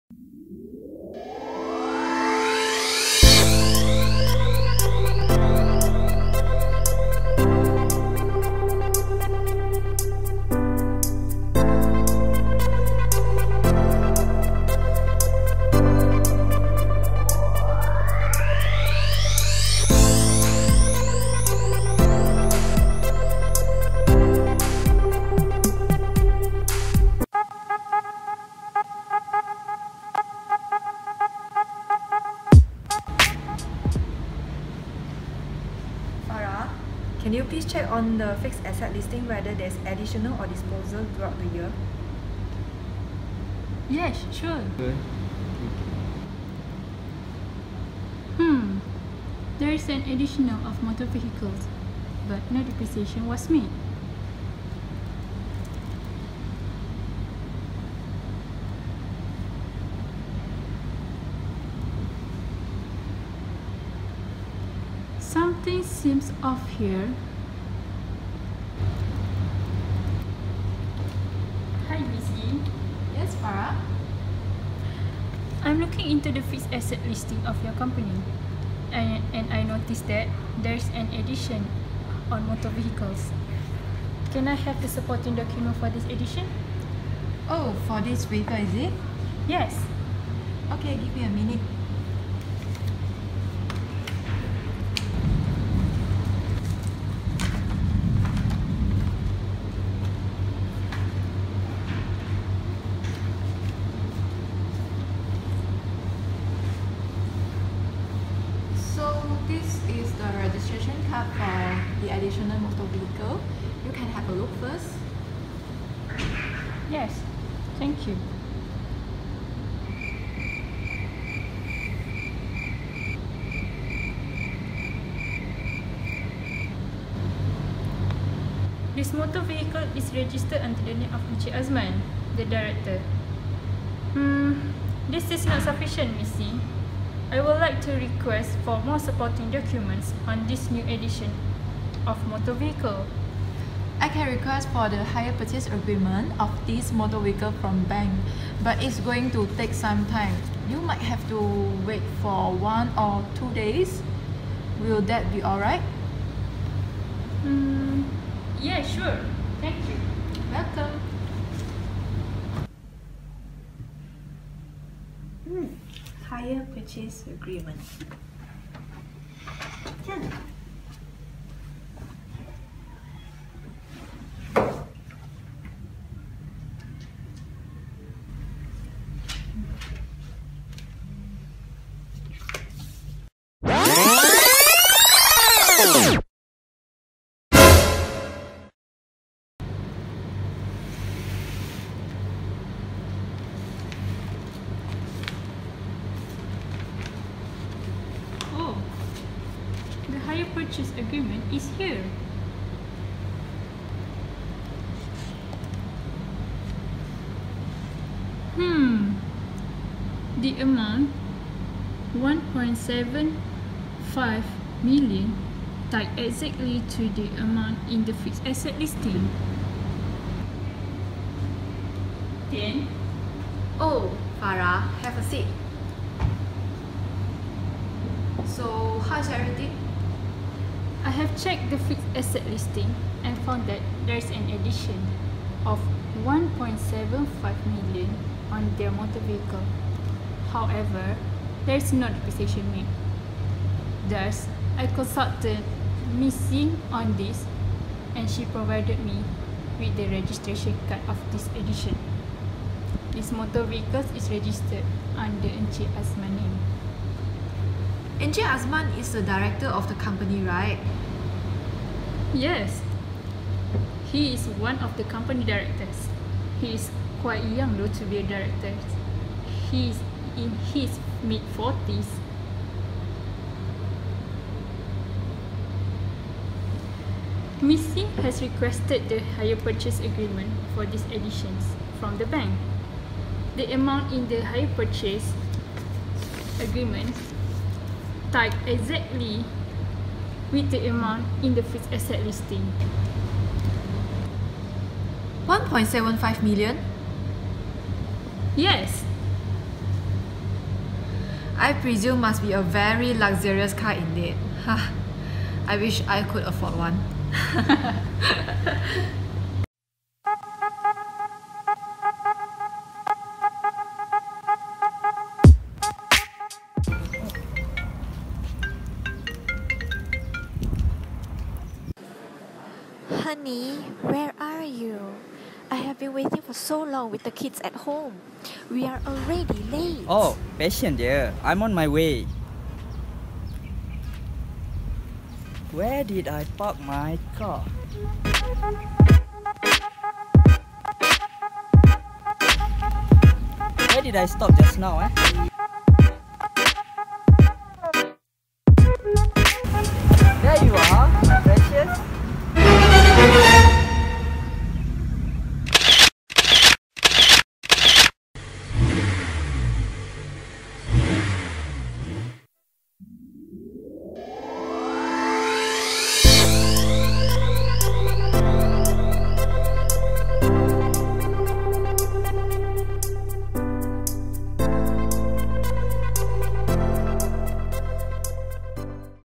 PutraMOOC || Property, Plant and Equipment : Was there any material misstatements?
This is a video acted out by students to illustrate the case on auditing property, plant and equipment specifically motor vehicle. Was there any material misstatements in this case?